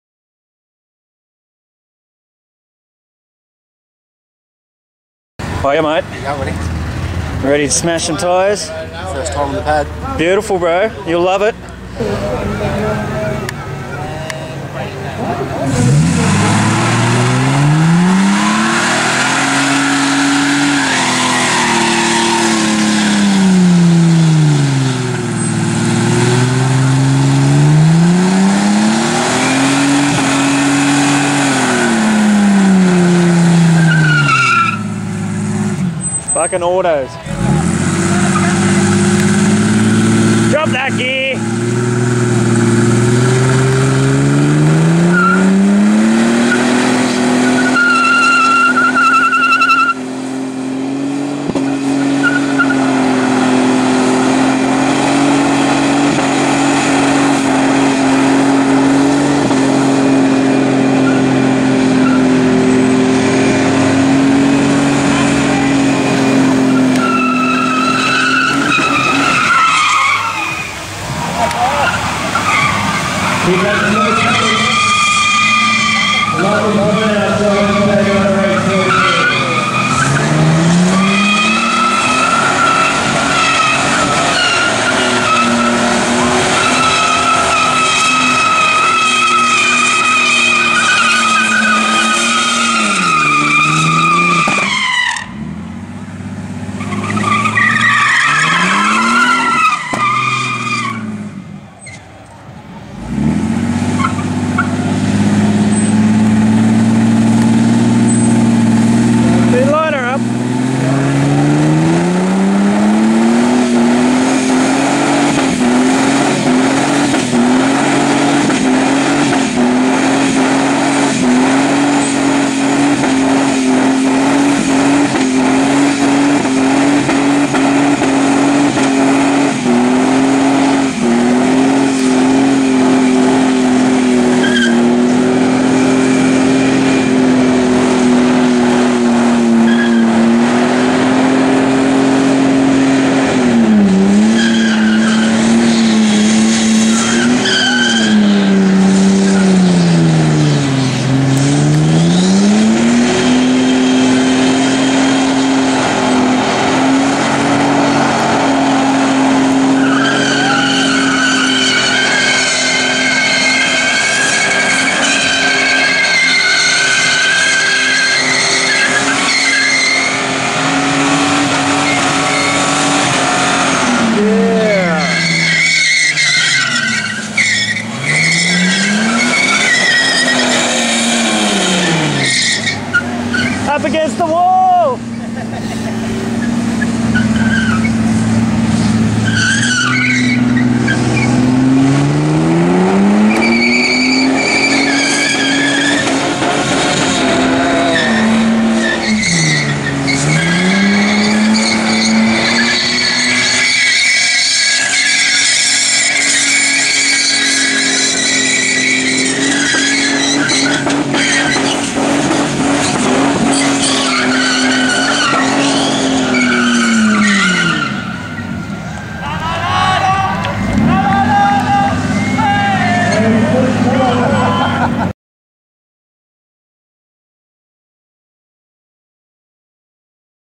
white au 23/10/2013
first timer to the pad and what a awesome job he did,the worst ford model ever made was given a beating,specially that auto lol,once it picked up it done a great job,cant wait to c u out there again mate